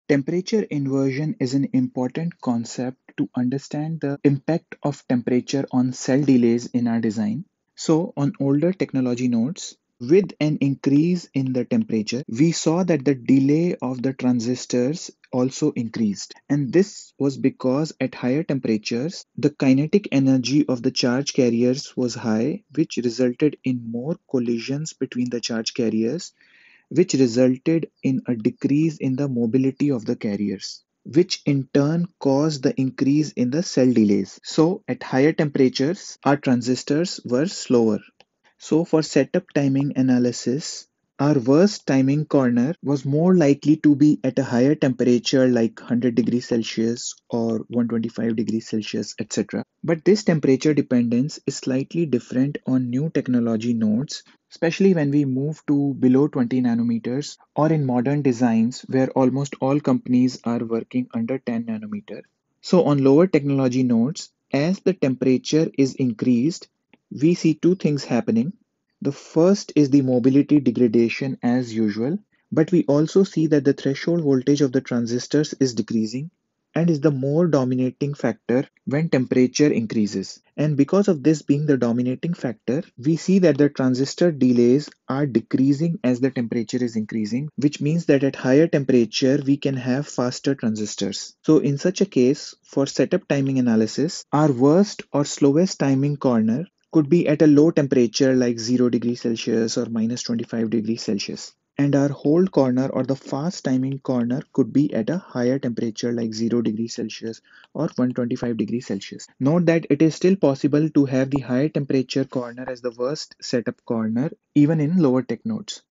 Temperature inversion | VLSI Interview prep | Physical design | Semiconductor 101 | Digital logic
Temperature inversion is a phenomenon observed in modern VLSI technologies where the delay of a transistor does not always increase with temperature, as was the case in older technology nodes. Traditionally, higher temperature reduces carrier mobility, which lowers the drive current and increases delay, while lower temperature improves mobility and makes transistors faster.

However, in advanced sub-65 nm and FinFET nodes, especially at lower supply voltages, another effect becomes significant: the threshold voltage (Vt) decreases with increasing temperature. This reduction in Vt makes it easier to turn the transistor on and increases the drive current. When supply voltage is low, the positive impact of reduced Vt outweighs the negative impact of reduced mobility, so transistors can actually switch faster at higher temperatures. This counterintuitive effect is called temperature inversion. The practical implication is that timing analysis can no longer assume worst-case setup delay at high temperatures and worst-case hold delay at low temperatures, as was done traditionally. Instead, both cold and hot corners must be analyzed carefully, since in some cases the cold condition becomes the slowest.